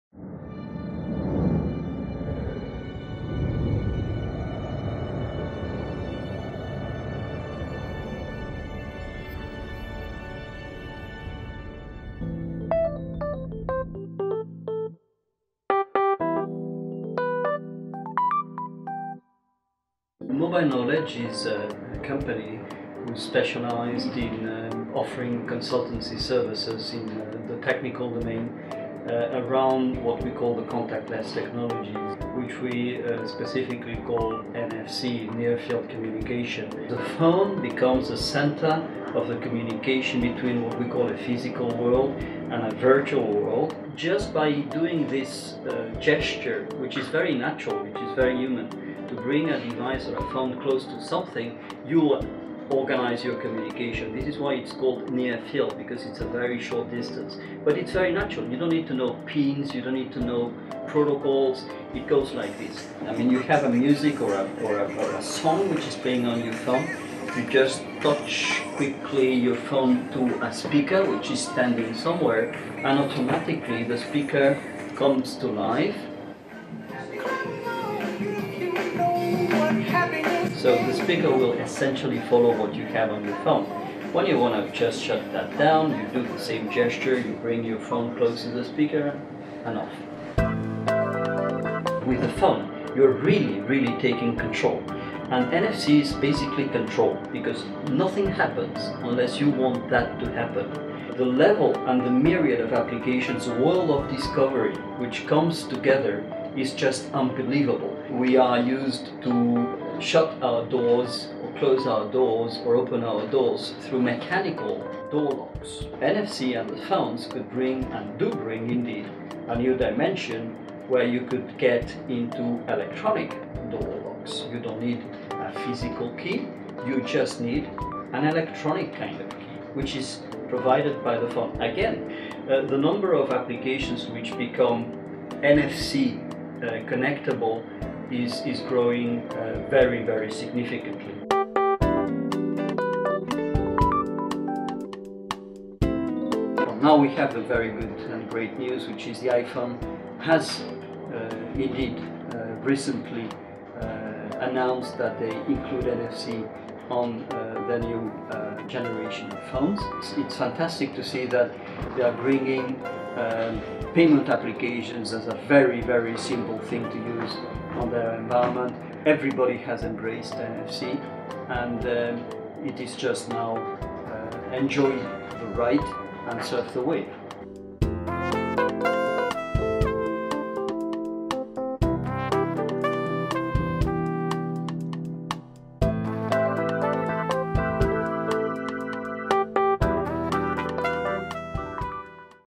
Mobile Knowledge, experts in NFC
Mobile Knowledge is a spin-off of the multinational NXP semiconductors focused on Near Field Technology. Based in Barcelona, in the same office of Mobile World Capital Barcelona Foundation, they offer consultancy worldwide about NFC projects, as well as training courses